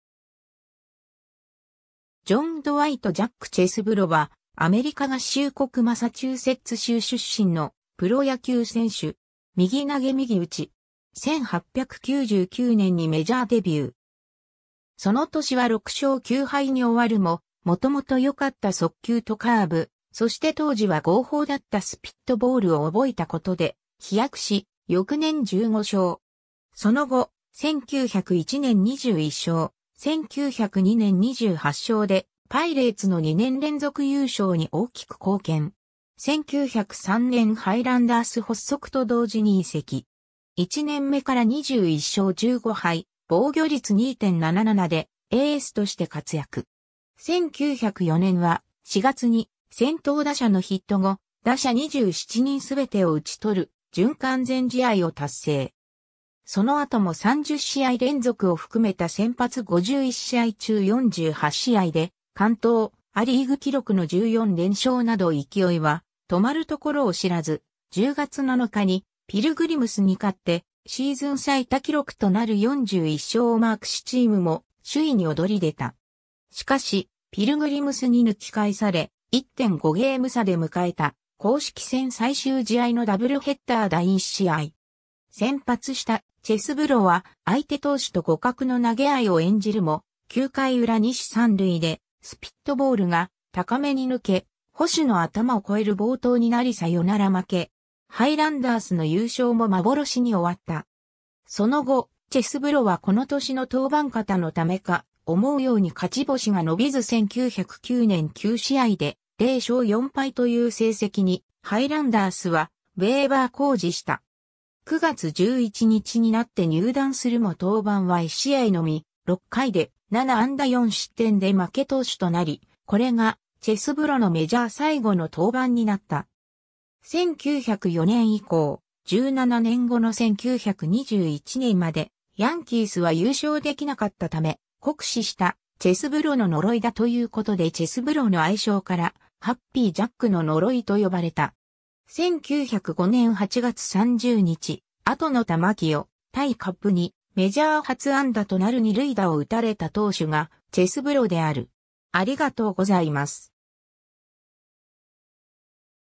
ジャック・チェスブロ
ジョン・ドワイト・"ジャック"・チェスブロ（John Dwight "Jack" Chesbro, 1874年6月5日 - 1931年11月6日）は、アメリカ合衆国マサチューセッツ州出身のプロ野球選手（投手）。
 1899年にメジャーデビュー。
その年は6勝9敗に終わるも、もともと良かった速球とカーブ、そして当時は合法だったスピットボールを覚えた事で飛躍し、翌年15勝。
その後、1901年21勝、1902年28勝でパイレーツの2年連続優勝に大きく貢献。
 1903年 ハイランダース発足と同時に移籍。
1年目から21勝15敗、防御率2.77でエースとして活躍。
 1904年は、4月に先頭打者のヒット後、打者27人全てを打ち取る準完全試合を達成。
その後も30試合連続を含めた先発51試合中48試合で完投、ア・リーグ記録（当時）の14連勝など勢いは止まるところを知らず、10月7日にピルグリムスに勝ってシーズン最多記録（20世紀以降）となる41勝をマークしチームも首位に躍り出た。
しかしピルグリムスに抜きかえされ、1.5ゲーム差で迎えた公式戦最終試合のダブルヘッダー第1試合。
先発したチェスブロは相手投手と互角の投げ合いを演じるも、9回裏二死三塁でスピットボールが高めに抜け、捕手の頭を超える暴投になりサヨナラ負け。
ハイランダースの優勝も幻に終わった。
 その後、チェスブロはこの年の登板過多のためか、思うように勝ち星が延びず1909年9試合で0勝4敗という成績にハイランダースはウェイバー公示した。
 9月11日になって入団するも登板は1試合のみ、6回で7安打4失点で負け投手となり、これがチェスブロのメジャー最後の登板になった。
 1904年以降、17年後の1921年までヤンキース（ハイランダース）は優勝できなかったため、「酷使したチェスブロの呪いだ」ということでチェスブロの愛称からハッピー・ジャックの呪いと呼ばれた。
 1905年8月30日、後の球聖，タイ・カッブにメジャー初安打となる二塁打を打たれた投手がチェスブロである。